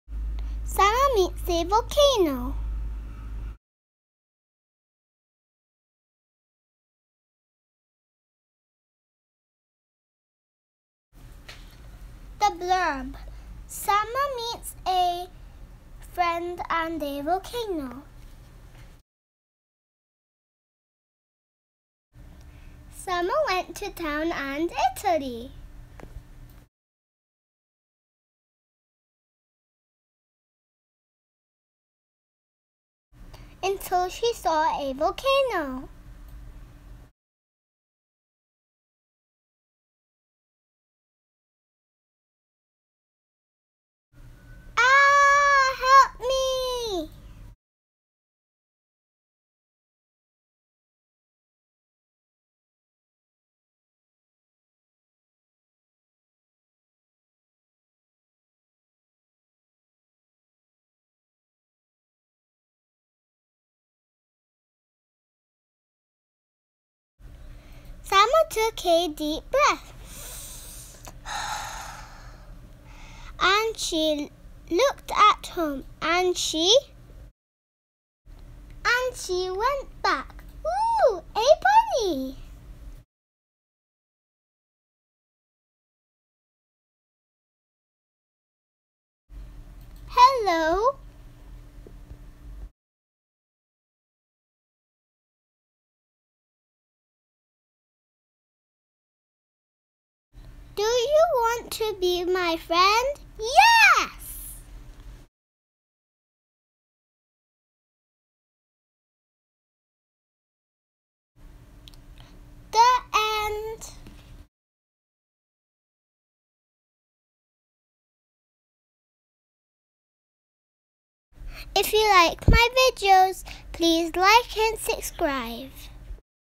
Sama meets a volcano Audiobook | Stories for children | Written and read by Zarah
Second book of the Sama's stories series

Written and read by 5 year old Zarah